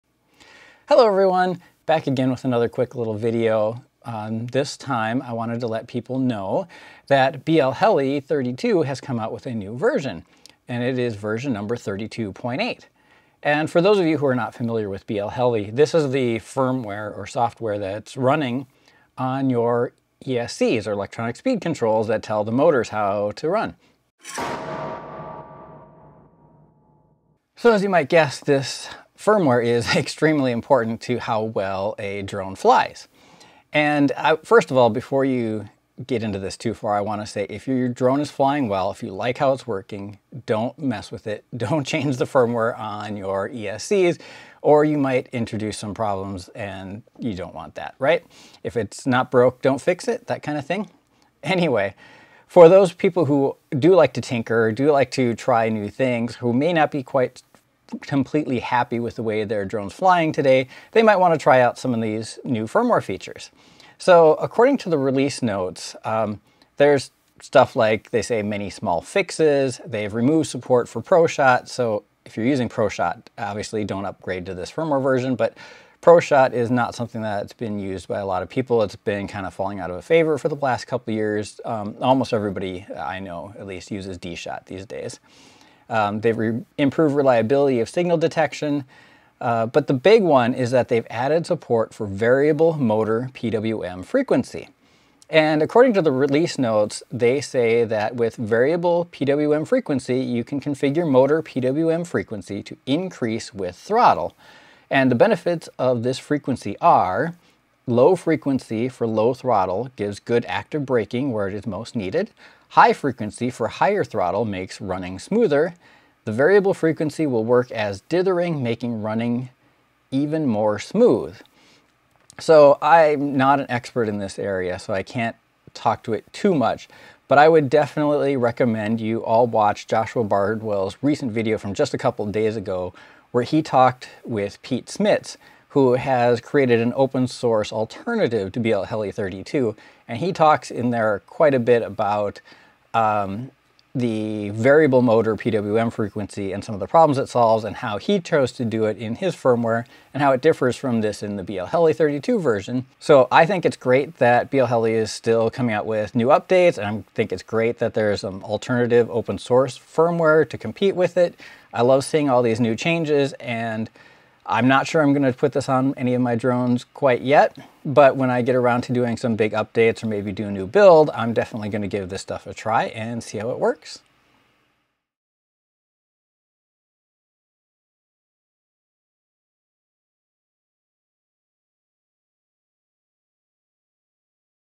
BlHeli32 Release 32.8 with Variable Motor PWM Frequency
A quick announcement that BLHeli32 has a new version with some small fixes, some dropped features, and one major improvement, variable motor PWM frequency.